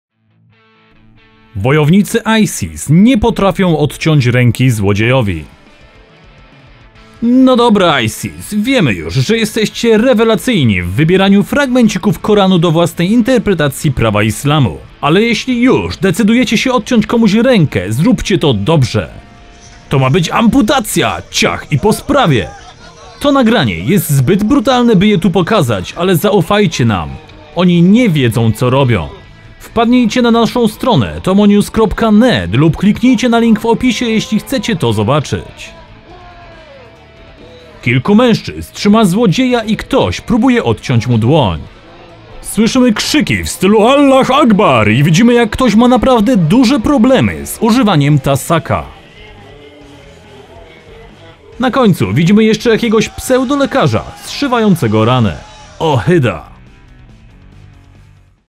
Wojownicy ISIS nie potrafią odciąć ręki złodziejowi
No dobra ISIS wiemu już, że jesteście rewelacyjnie w wybieraniu fragmencikó Koranu do własnej interpretacji prawa Islamu, ale jeśli już decydujecie się odciąć komuś rękę, zróbice to dobrze!
To ma być amputacja! Ciach i po sprawie!
To nagranie jest zbyt brutalne by je tu pokazać, ale zaufajcie nam, onie nie wiedzą co robią.
Wpadnijcie na naszą stronę tomonews.net lub kliknijcie na link w opisie jeśli chcecie to zobaczyć.
Kilku mężczyzn trzyma złodzieja i ktoś próbuje odciąć mu dłoń. Słyszymy krzyki w stylu Allah Akbar i widzimy jak ktoś naprawdę ma spore problemy z używaniem tasaka.
Na końcu widzimy jeszcze jakiegoś pseudolekarza zaszywającego ranę...ohyda!